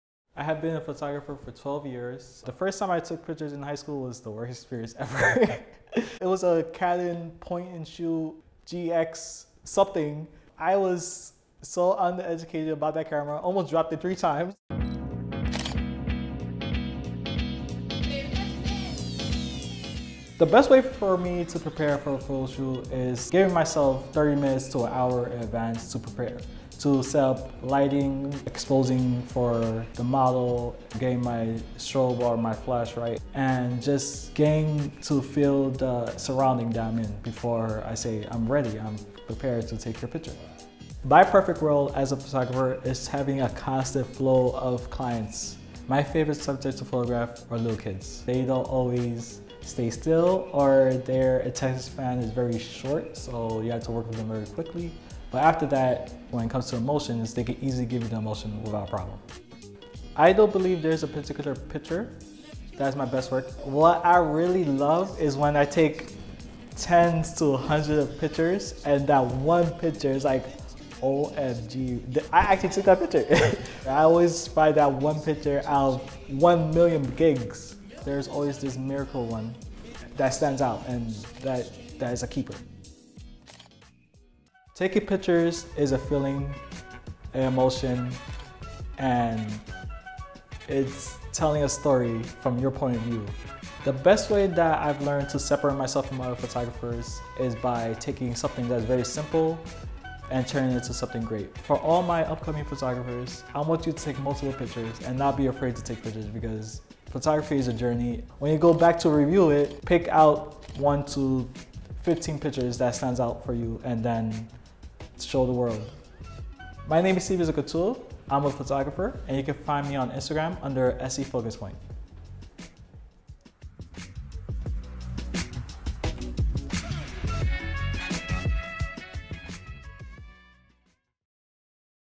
Pinkroommedia Spotlight Series: SC Focus Point (2018)
New York City photographer Stevenson Catul opens up about his early days as a photographer and gives advice on how to break into the industry.